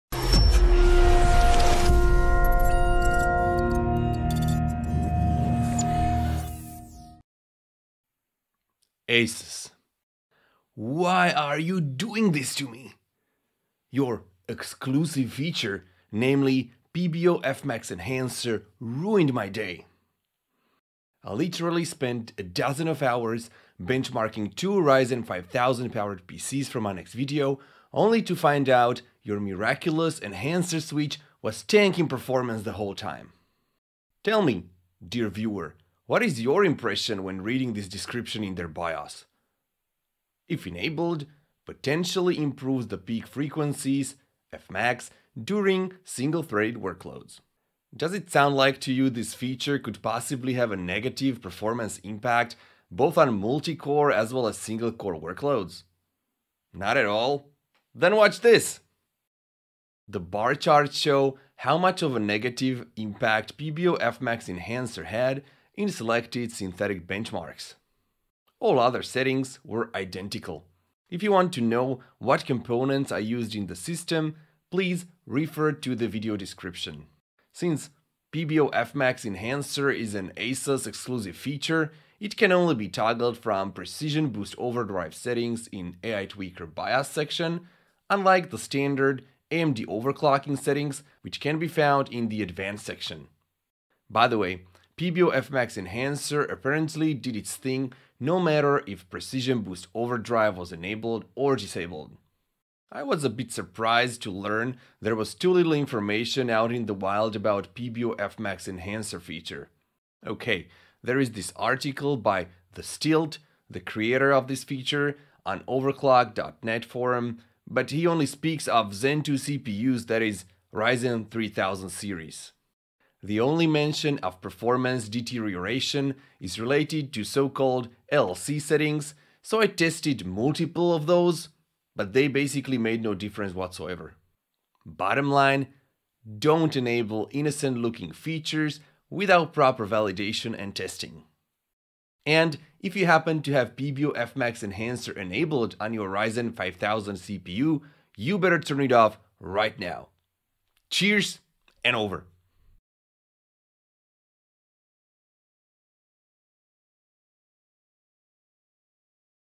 Asus PBO Fmax Enhancer FAIL
In Q4/2020, Asus introduced a new toggle, PBO Fmax Enhancer, in their UEFI BIOS for their AM4 motherboards.
It was supposed to boost single core performance but it turned out, it is actually hurting it, at least when paired with AMD Ryzen 5000 series processors.

I used the following two systems for testing:

### 1st PC ###
Mobo: Asus TUF Gaming X570-Pro WiFi
CPU: AMD Ryzen 7 5800X
Cooler: Asus TUF LC 240 AIO
RAM: 32GB G.Skill DDR-3733 CL14 (Samsung B-die)
PSU: Asus ROG Strix 850W Gold

### 2nd PC ###
Mobo: Asus Strix X570-E Gaming WiFi
CPU: AMD Ryzen 5 5600X
Cooler: Noctua NH-D15S
RAM: 32GB Kingston Fury DDR4-3633 CL16
PSU: EVGA 650GQ Gold